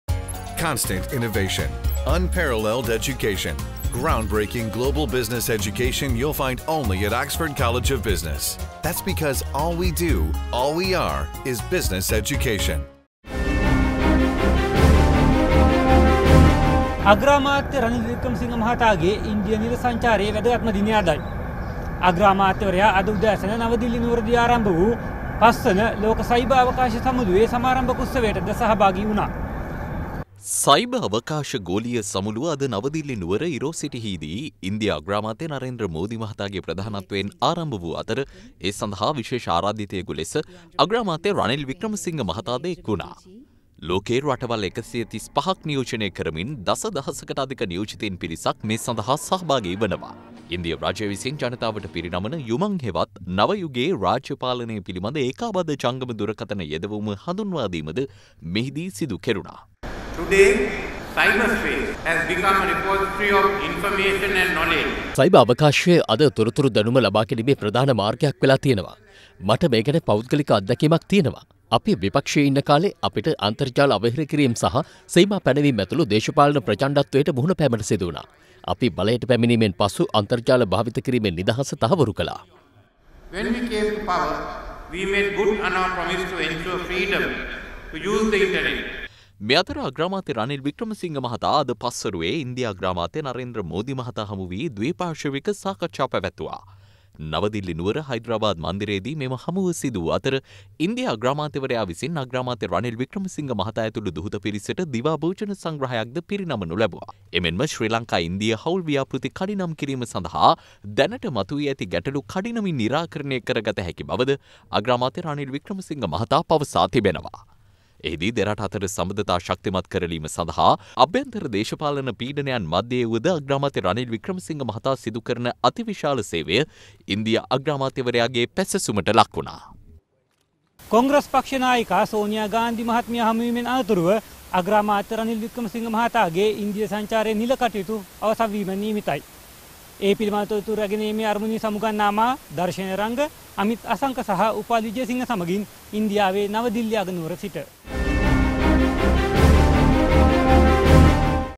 Modi meets visiting PM Ranil Wickremesinghe
Indian Prime Minister Narendra Modi on Thursday met visiting
Sri Lankan Premier Ranil Wickremesinghe in New Delhi,
officials said.

සයිබර් අවකාශ ගෝලීය සමුළුව විවෘත කිරීමේ උළෙලට විශේෂ ආරාධිතයෙකු
වශයෙන් ශ්‍රී ලංකා අග්‍රාමාත්‍ය රනිල් වික්‍රමසිංහ මහතා අද සහභාගී විය.